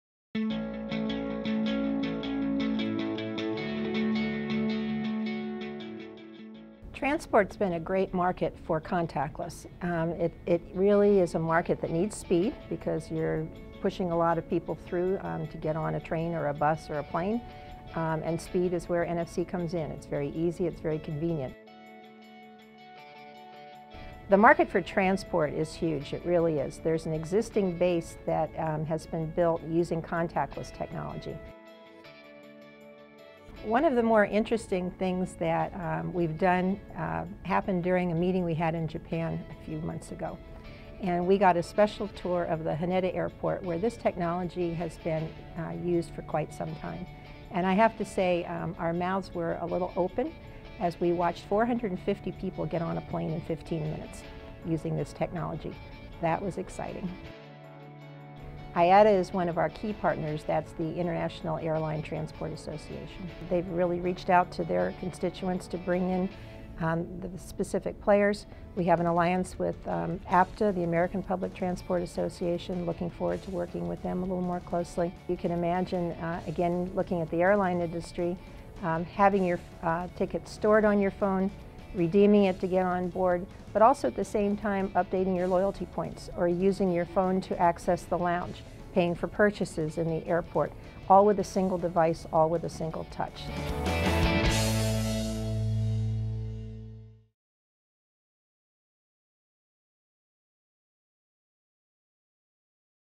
TRANSPORT
NFC Forum Director Debbie Arnold provides insight into the many applications for NFC in public transit, air travel, and related areas.